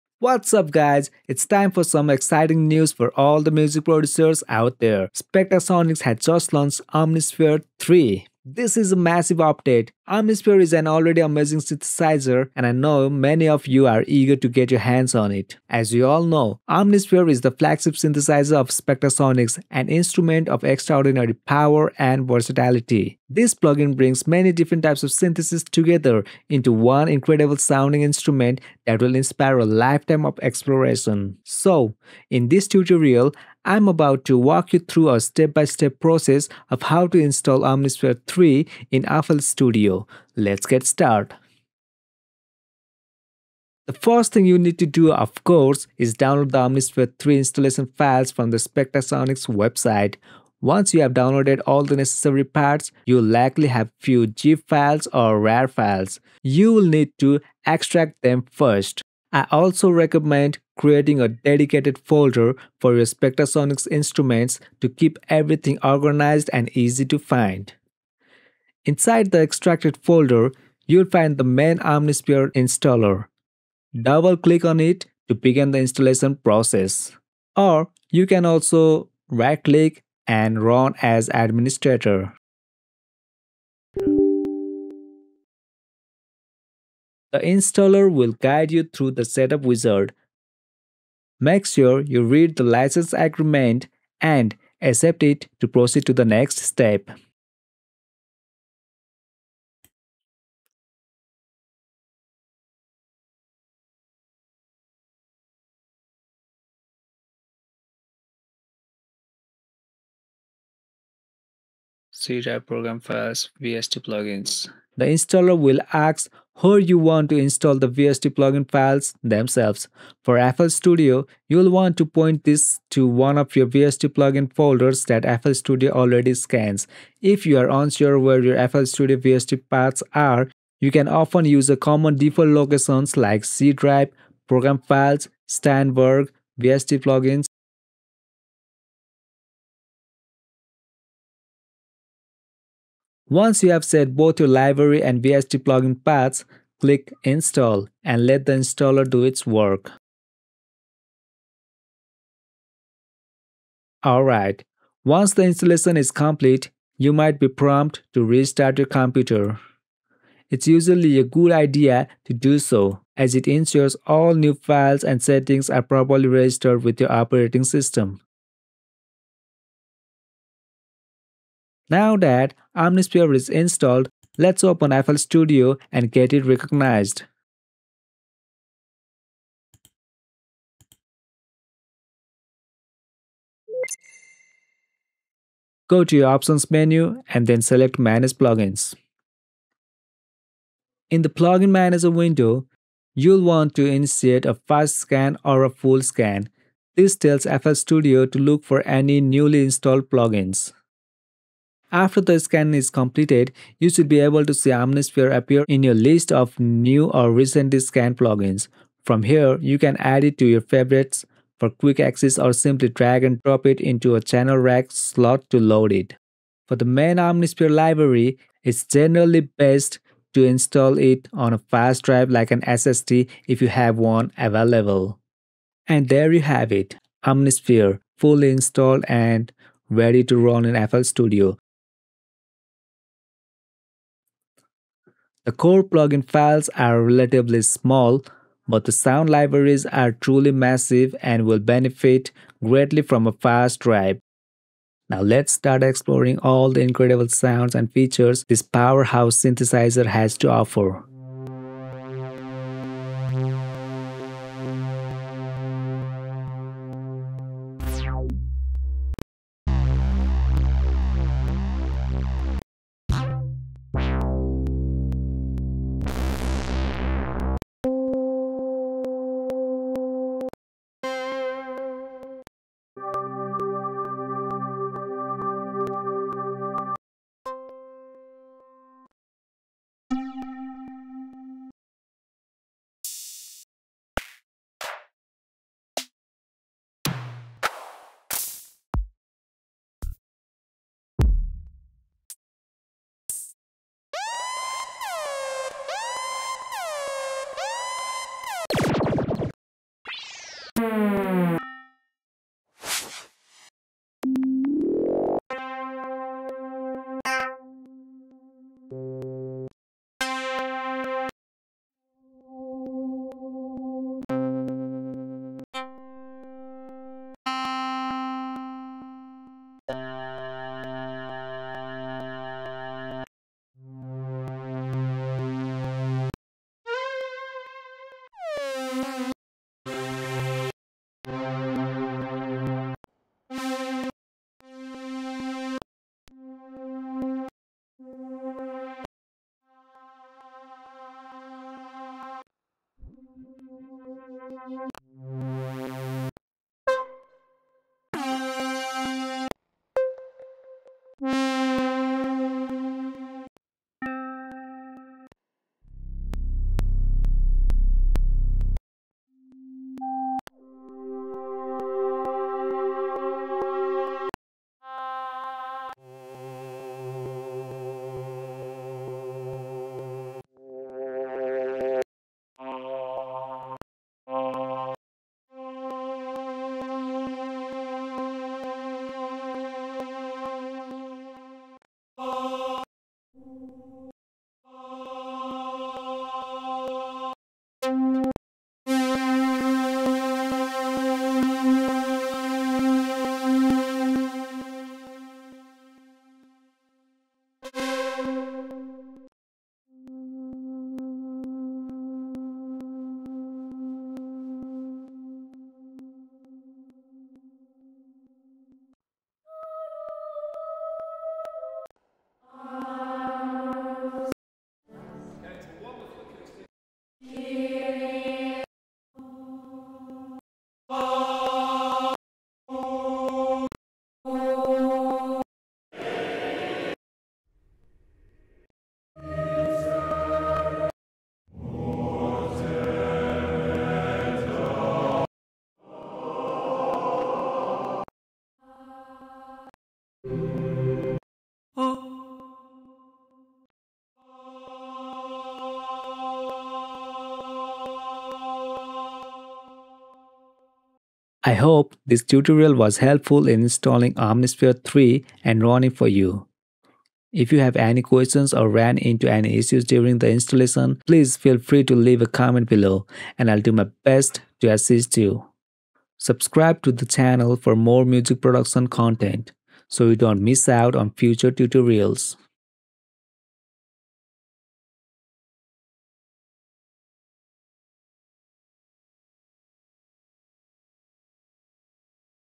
How To Install Spectrasonics Omnisphere 3 in Fl Studio | The Easiest Way
Step by Step Process of How To Install Spectrasonics Omnisphere 3 in Fl Studio, The Easiest Way !!